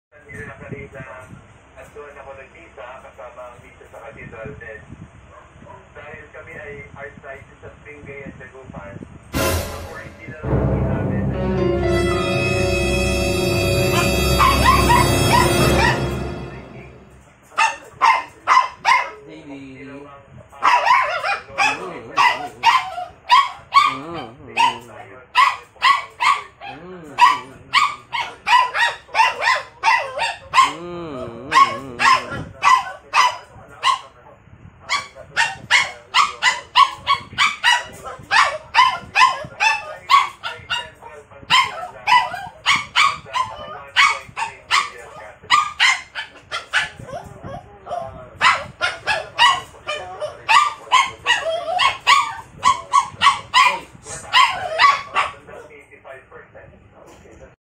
Jealous Poodle
My poodle got extremely jealous with a stuff toy.